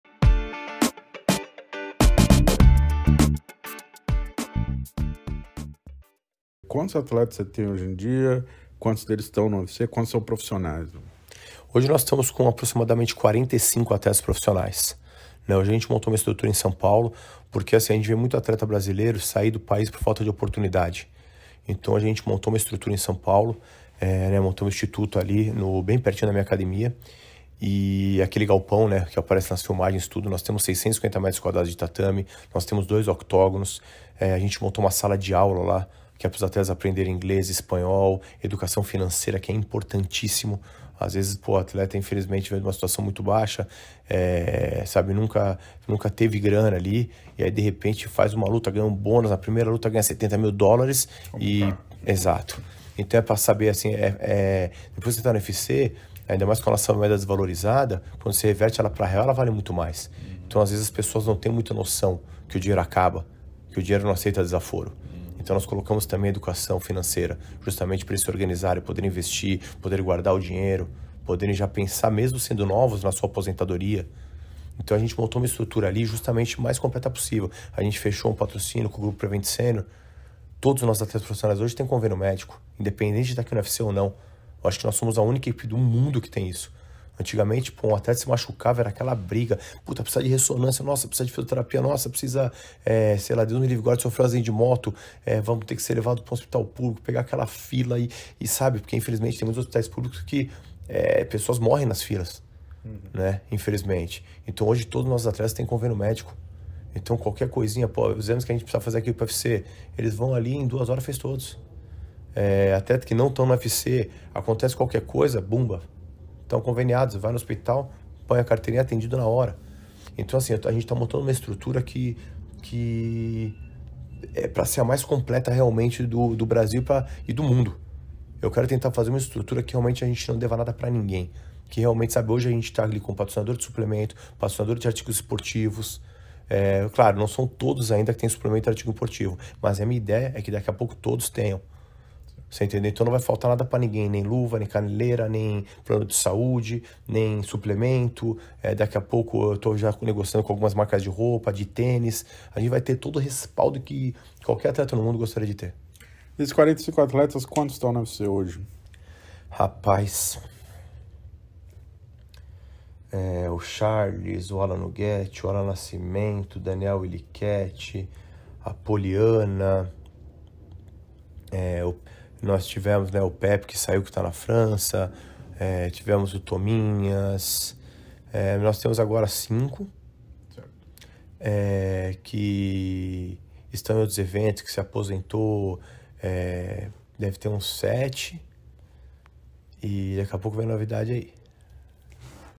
Why is Charles Oliveira's coach so smart? Know the different method of his academy. (subtitled)
This interview was granted to the channel Ag.Fight, one of the most respected news agencies in Brazil in the MMA segment. Here you can access the full episode

Diego Lima is a coach at Chute Boxe and trains Charles Oliveira, UFC lightweight champion. Diego talks a little about his differentiated view of training with high performance athletes.

Intro sound credits -